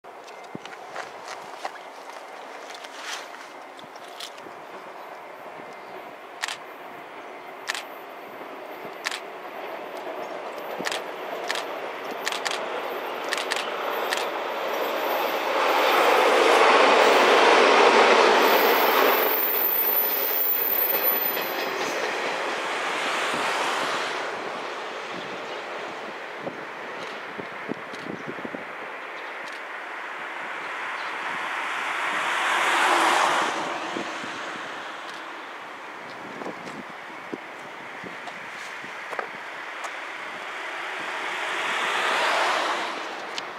一部退役へ！あいの風とやま鉄道413系
JR西日本七尾線の413系が3月ダイヤ改正で引退したばかりですが、今度はあいの風とやま鉄道所属413系も一部が退役することになりました。5月5日には、富山⇔泊駅間を廃車となる車両を用いた6両編成を特別ダイヤを運行する予定です。

00063